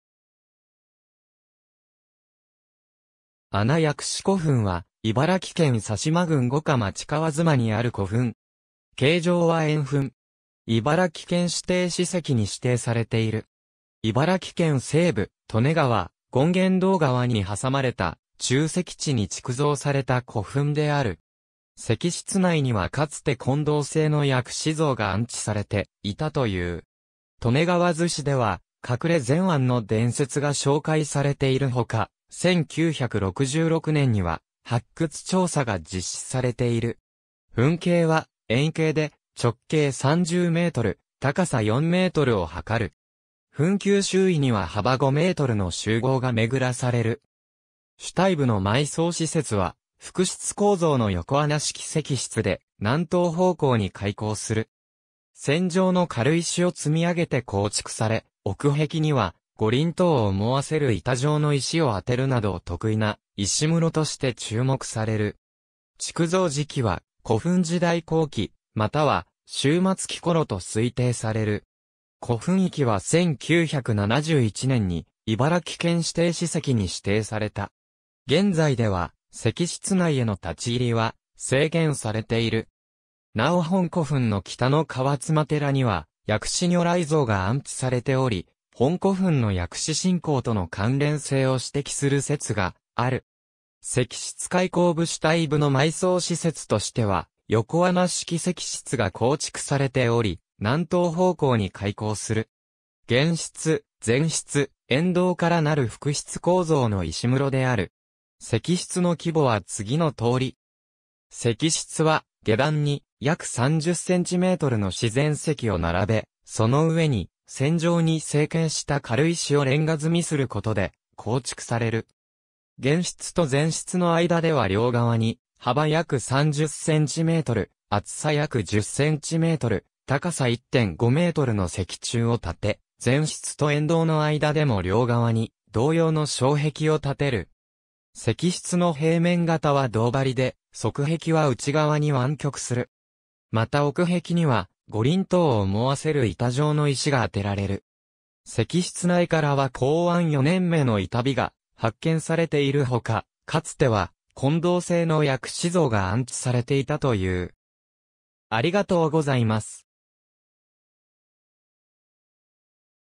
穴薬師古墳 (五霞町)
穴薬師古墳（あなやくしこふん）は、茨城県猿島郡五霞町川妻にある古墳。
形状は円墳。
茨城県指定史跡に指定されている。
茨城県西部、利根川・権現堂川に挟まれた沖積地に築造された古墳である。
石室内にはかつて金銅製の薬師像が安置されていたという。
『利根川図志』では「隠れ膳椀」の伝説が紹介されているほか、1966年（昭和41年）には発掘調査が実施されている。
墳形は円形で、直径30メートル・高さ4メートルを測る。
墳丘周囲には幅5メートルの周濠が巡らされる。
主体部の埋葬施設は複室構造の横穴式石室で、南東方向に開口する。
磚状（レンガ状）の軽石を積み上げて構築され、奥壁には五輪塔を思わせる板状の石をあてるなど特異な石室として注目される。
築造時期は古墳時代後期または終末期頃と推定される。
古墳域は1971年（昭和46年）に茨城県指定史跡に指定された。
現在では石室内への立ち入りは制限されている。
なお本古墳の北の川妻寺には薬師如来像が安置されており、本古墳の薬師信仰との関連性を指摘する説がある。
石室開口部 主体部の埋葬施設としては横穴式石室が構築されており、南東方向に開口する。
玄室・前室・羨道からなる複室構造の石室である。
石室は、下段に約30センチメートルの自然石を並べ、その上に磚状に整形した軽石をレンガ積みすることで構築される。
玄室と前室の間では両側に幅約30センチメートル・厚さ約10センチメートル・高さ1.5メートルの石柱を立て、前室と羨道の間でも両側に同様の障壁を立てる。
石室の平面形は胴張りで、側壁は内側に湾曲する。
また奥壁には五輪塔を思わせる板状の石があてられる。
石室内からは弘安4年（1281年）銘の板碑が発見されているほか、かつては金銅製の薬師像が安置されていたという。